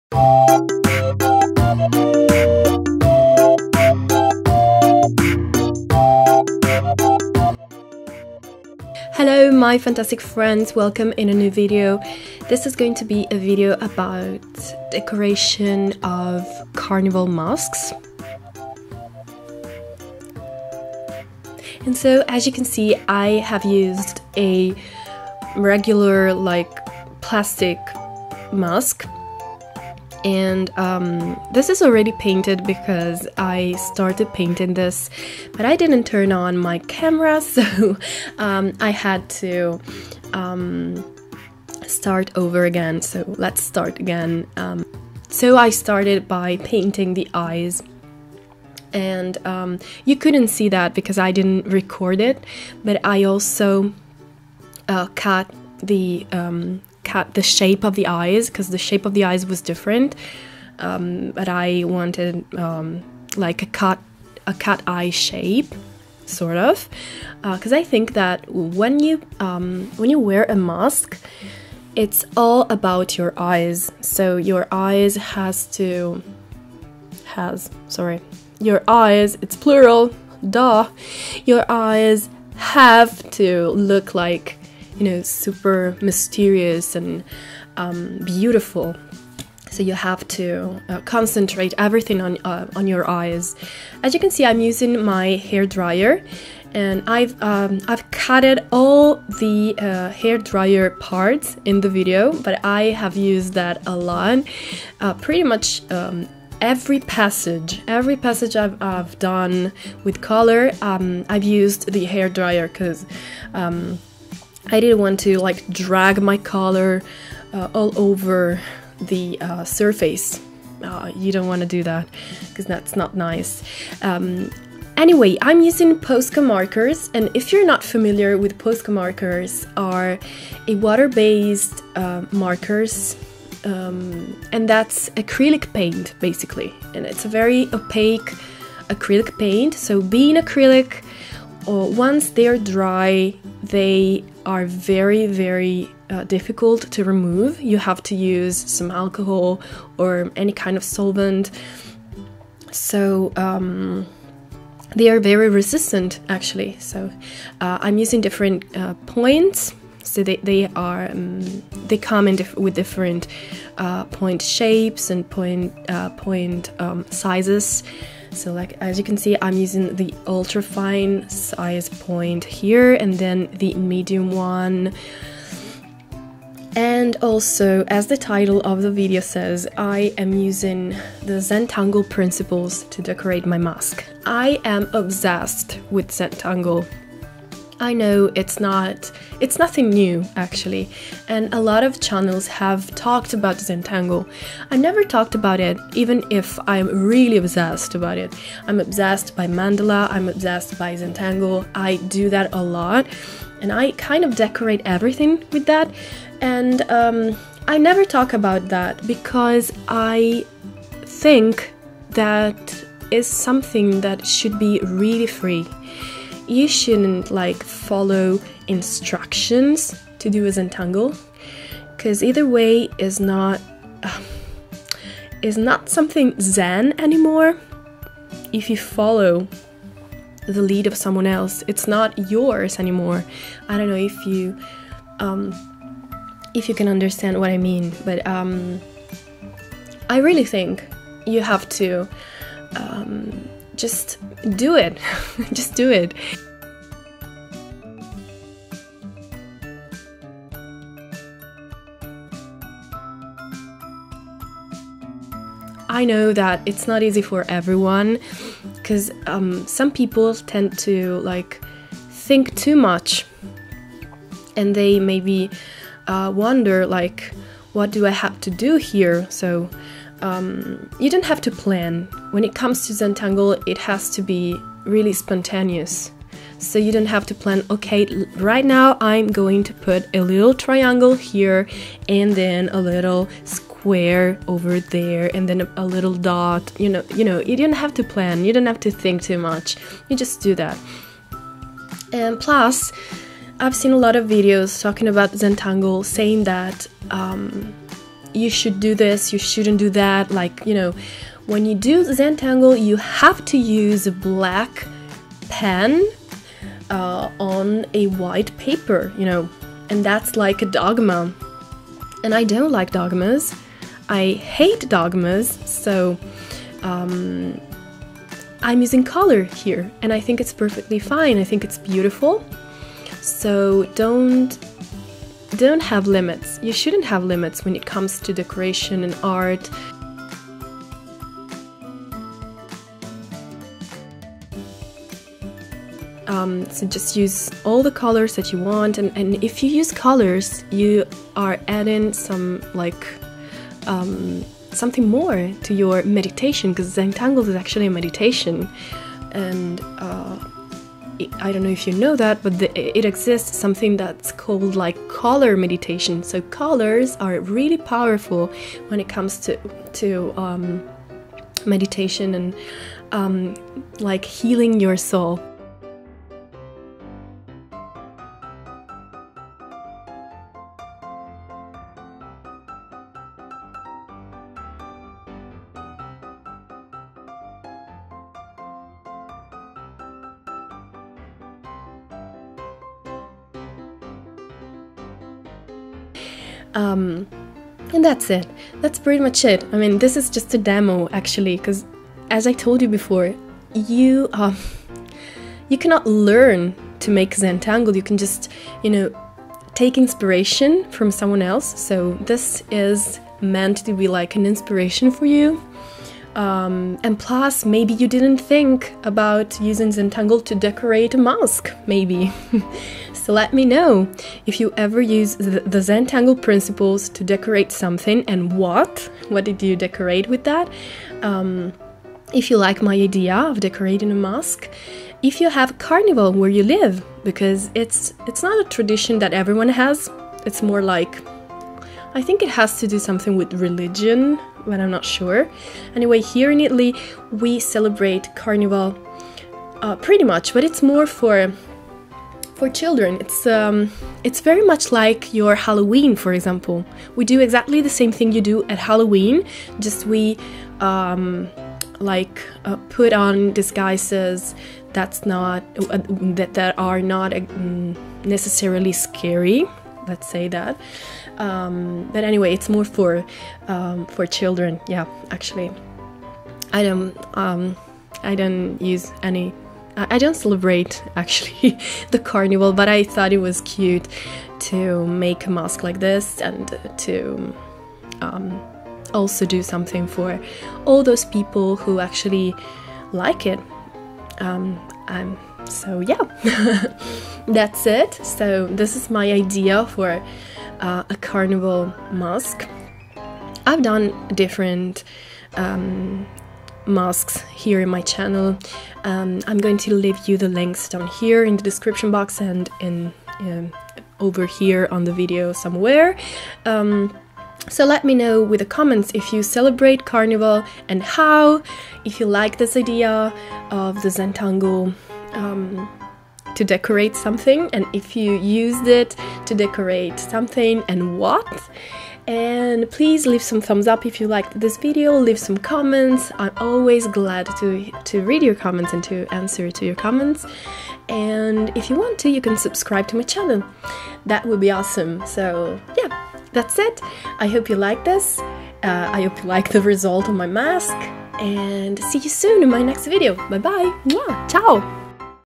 ZENTANGLE mask decorated with POSCA markers by ART Tv
Hello my fantastic friends welcome in a new diy tutorial in this video we are going to be decorating a mask for carnival (or halloween or any masquerade event) using POSCA MARKERS by Osama spa.
Posca markers are acrylic water based colors which are super resistant. They have an high coverage and an opaque finish.
The colors are bright and rich.
Perfect to decorate any surface.
For this diy idea I personalized a regular plastic mask (bought for 0.80 euro cent), decorated with ZENTANGLE principles using colors.
So this is not only a decoration but also a powerful meditation and a moment you take to relaz yourself and exercize at the same time.